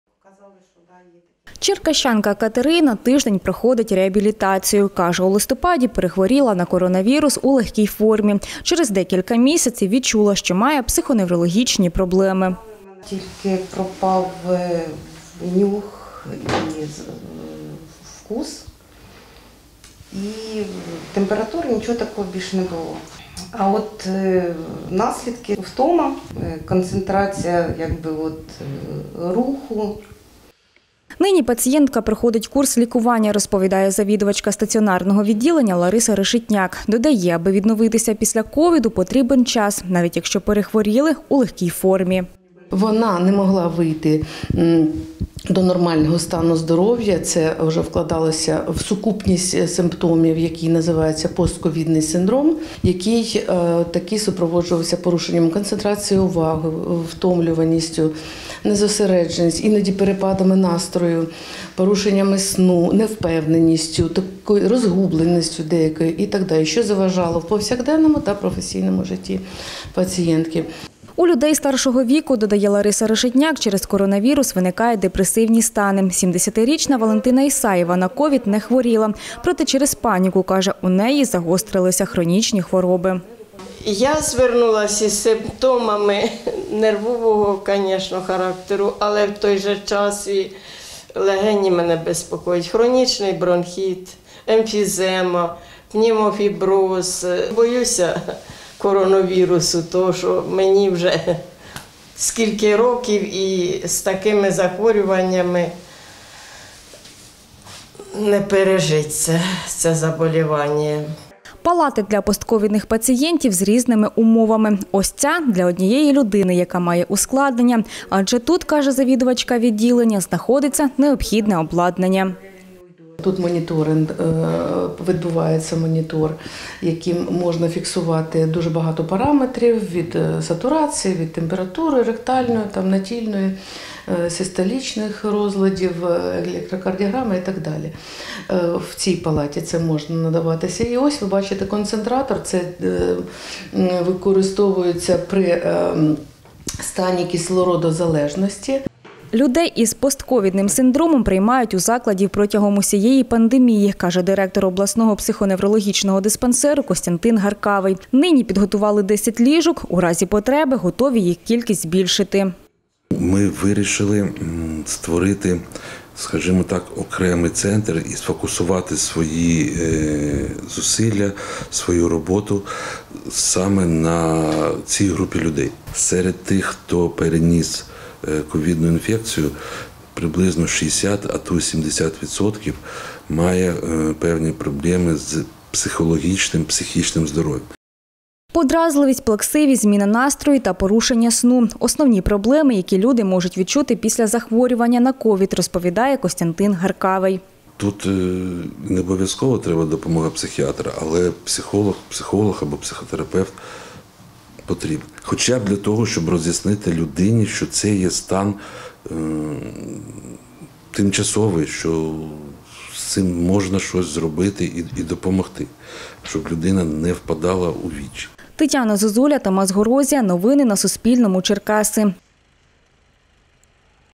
Реабілітація після коронавірусу у психоневрологічному диспансері
Шістьом пацієнтам із ускладненнями після коронавірусу надають допомогу на базі обласного психоневрологічного диспансеру. З 11 січня тут обладнали палати для реабілітації. Нині є десять ліжок.